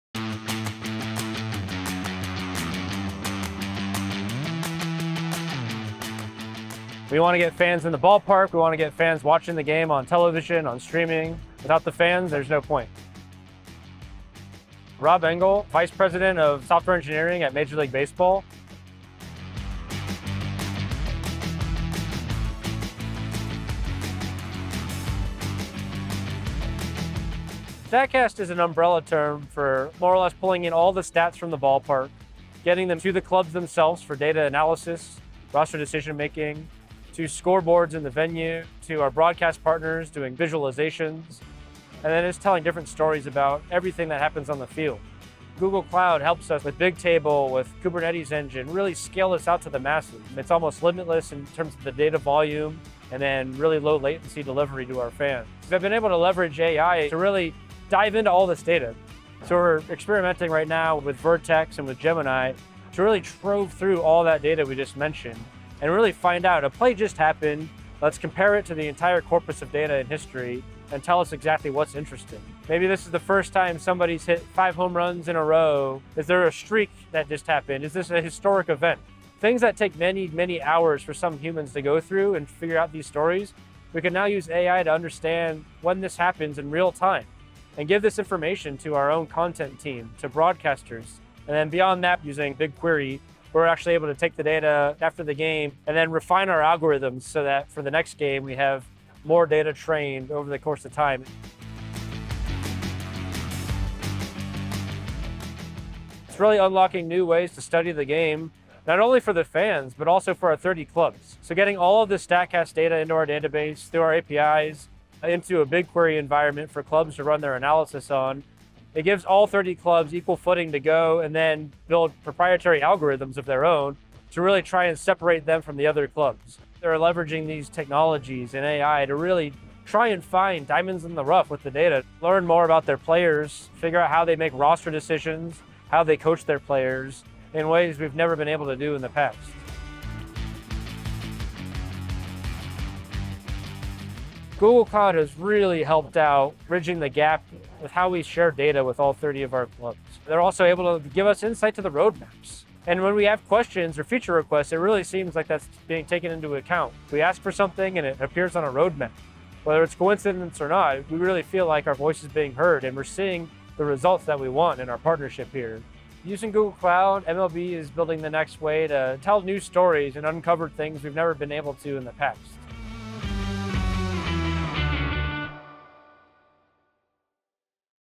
New Way Now: MLB knocks fan experience out of the park with Google Cloud
𝗦𝘂𝗺𝗺𝗮𝗿𝘆: Rob Engel, vice president of software engineering at Major League Baseball (MLB), shares how Google Cloud helped the professional sports league tap into AI to deliver data-driven game and fan experiences. With Google Cloud, MLB™ is transforming its trove of ballpark data into homerun-worthy stories that bring the live action on the field to life for fans, teams, and broadcast partners.

𝗖𝗵𝗮𝗹𝗹𝗲𝗻𝗴𝗲: How can MLB uncover the best insights from the data captured with Statcast — its state-of-the-art baseball metrics tracking system — and share these exciting stories as they happen on the field?

𝗦𝗼𝗹𝘂𝘁𝗶𝗼𝗻: Using Vertex AI and Gemini, MLB can compare live plays with its massive Statcast dataset in real time and find fresh insights —  from record-breaking home run hits to perfect pitches and new streaks. MLB also delivers data from every game directly into BigQuery, enabling all 30 teams in the league to access it and build AI models that help them study their players, make roster decisions, and improve team performance.

𝗥𝗲𝘀𝘂𝗹𝘁𝘀: Now, MLB can find the most thrilling moments and stats happening during games to share with content teams, sports broadcasters, and fans in real time.

𝗛𝗶𝗴𝗵𝗹𝗶𝗴𝗵𝘁𝘀 𝗮𝗻𝗱 𝘁𝗮𝗸𝗲𝗮𝘄𝗮𝘆𝘀 𝗳𝗿𝗼𝗺 𝗚𝗼𝗼𝗴𝗹𝗲 𝗖𝗹𝗼𝘂𝗱’𝘀 𝗶𝗻𝘁𝗲𝗿𝘃𝗶𝗲𝘄 𝘄𝗶𝘁𝗵 𝗥𝗼𝗯 𝗘𝗻𝗴𝗲𝗹, 𝗩𝗣 𝗼𝗳 𝗦𝗼𝗳𝘁𝘄𝗮𝗿𝗲 𝗘𝗻𝗴𝗶𝗻𝗲𝗲𝗿𝗶𝗻𝗴 𝗮𝘁 𝗠𝗟𝗕:

→ “Google Cloud helps us with BigTable, with Kubernetes Engine, really scale Statcast out to the masses. It's almost limitless in terms of the data volume and then really low latency delivery to our fans. And we’ve been able to leverage AI along with this to really dive into all this data.”

→ “We’re experimenting with Vertex AI and Gemini to really go through all the data. A play just happened — let’s compare it to the entire corpus of data in history and tell us exactly what’s interesting. Things that take many hours for some humans to go through and figure out these stories, we can now use AI to understand what happens in real time and give this information to our own content team, to broadcasters.”

→ “Using BigQuery, we’re actually able to take the data after the game and then refine our algorithms so that from the next game we have more data trained over the course of time. It’s really unlocking new ways to study the game, not only for the fans but also for our 30 clubs.”

→ “Google Cloud has really helped out bridging the gap of how we share data with all 30 of our clubs. They’re also able to give us insights to the roadmaps, and when we have questions or feature requests, it really seems like that's being taken into account. We really feel like our voice is being heard, and we’re seeing the results that we want in our partnership.”

→ “Using Google Cloud, MLB is building the next way to tell new stories and uncover things we’ve never been able to in the past.”

Google Cloud products used: Vertex AI, Gemini, BigQuery, BigTable, Google Kubernetes Engine MLB

Major League Baseball trademarks and copyrights are used with permission of Major League Baseball. Visit MLB.com